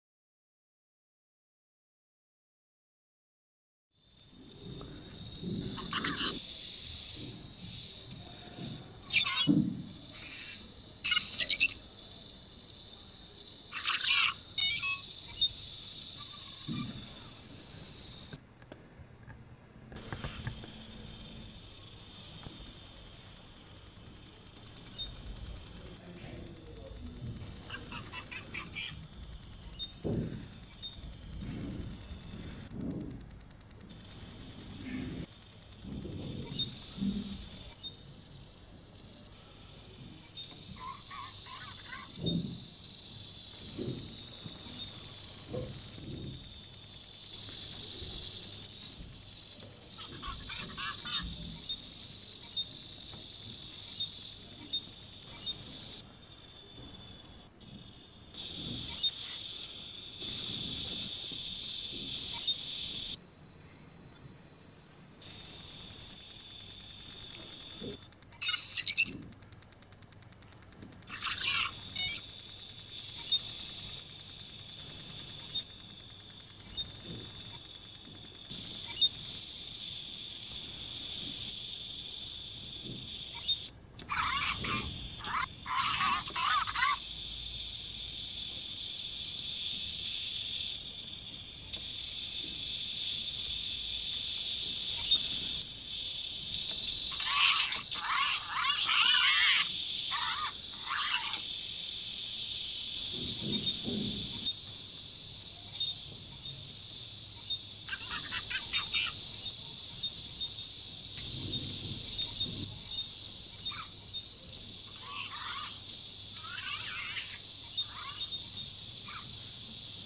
Ozobot Robot Physics Place Time Graph
The smallest robot Ozobot Evo used in physics lesson